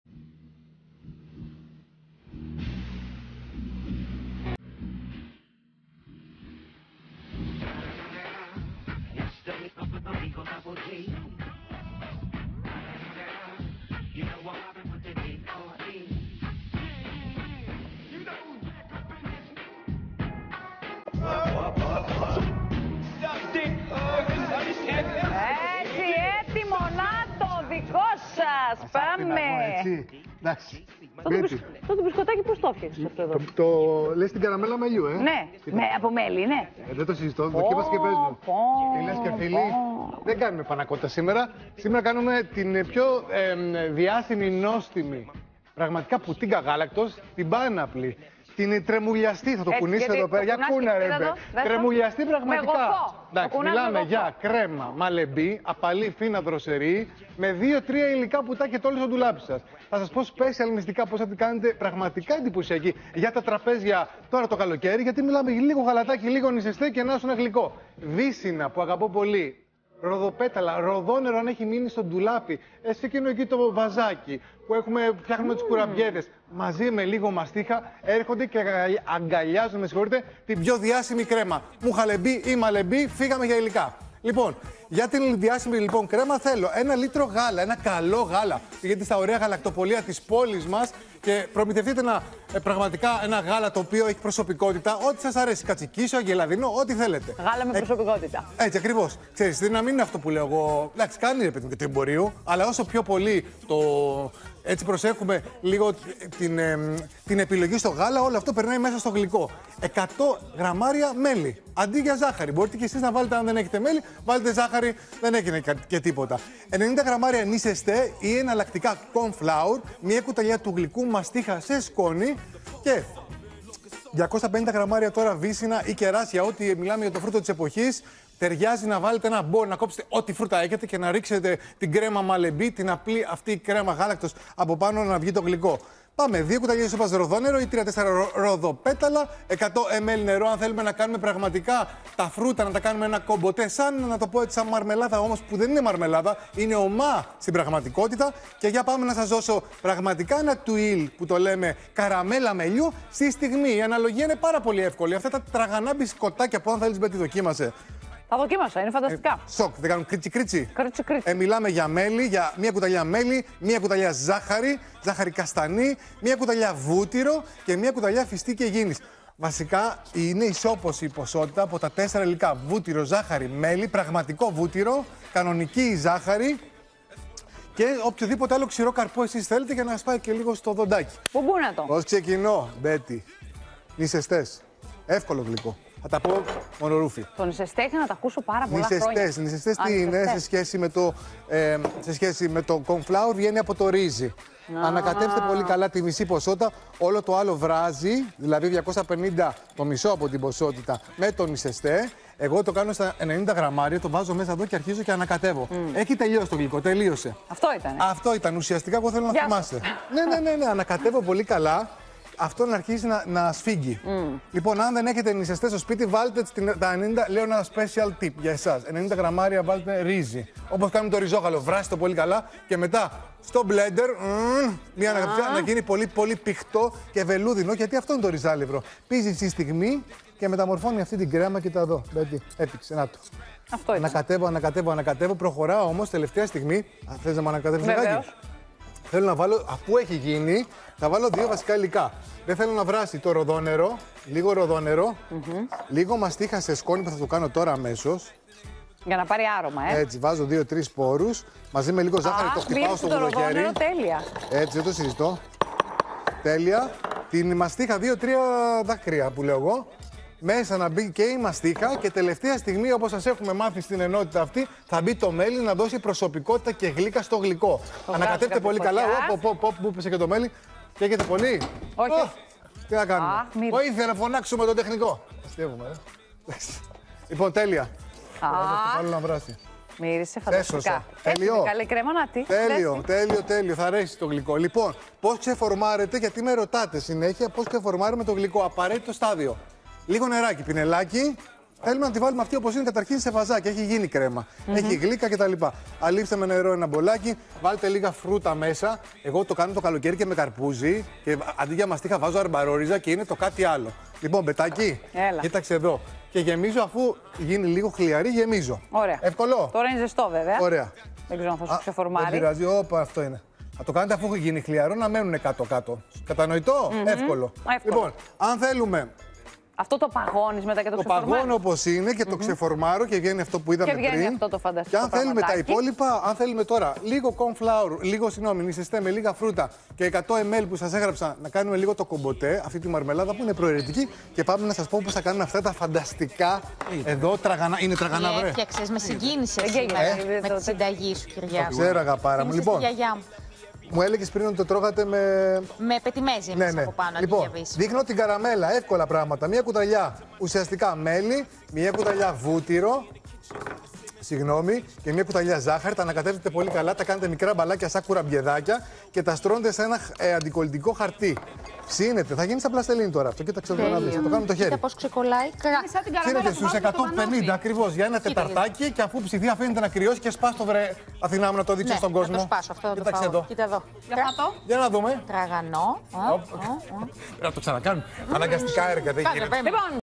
Ο Κυριάκος Μελάς φτιάχνει μαστιχένια κρέμα μουχαλεμπί με βύσσινα και τραγανή καραμέλα μελιού|OPEN TV
Γίνετε μέλος στο κανάλι του OPEN για να αποκτήσετε πρόσβαση σε προνόμια:
Συνταγή για μαστιχένια κρέμα μουχαλεμπί με βύσσινα και τραγανή καραμέλα μελιού από τον Κυριάκο Μελά. Υλικά συνταγής: 1 λίτρο γάλα, 100 γρ. μέλι, 90 γρ. νισεστέ /κορν φλάουερ, 1 κ.γ μαστίχα σε σκόνη, 250 γρ. βύσσινα/ κεράσια, 2 κ.σ ροδόνερο ή  3-4 ροδοπέταλα και 100 ml νερό. Για το σερβίρισμα: 2 κ.σ φιστίκι Αιγίνης. Για το τουίλ – καραμέλα: 20 γρ. μέλι 20 γρ. ζάχαρη καστανή, 20 γρ. βούτυρο και 20 γρ. φιστίκι Αιγίνης.